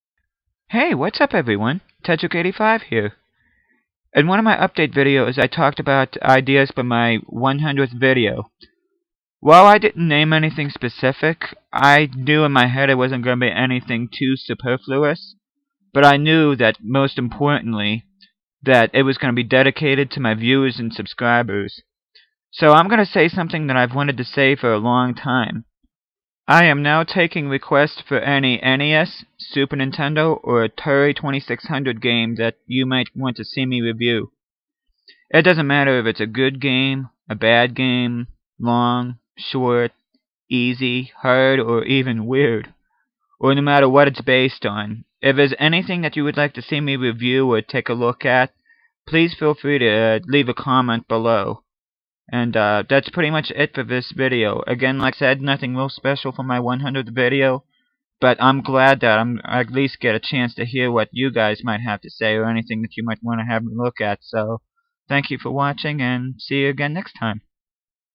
Viewers' choice reviews announcement
I am so happy that I got to make this short video and say what I've wanted to say since I first started my channel. I already have some reviews in mind to do, but if someone happens to suggest one that I have already thought of, I will still give them my thanks and recognition for the suggestion. :)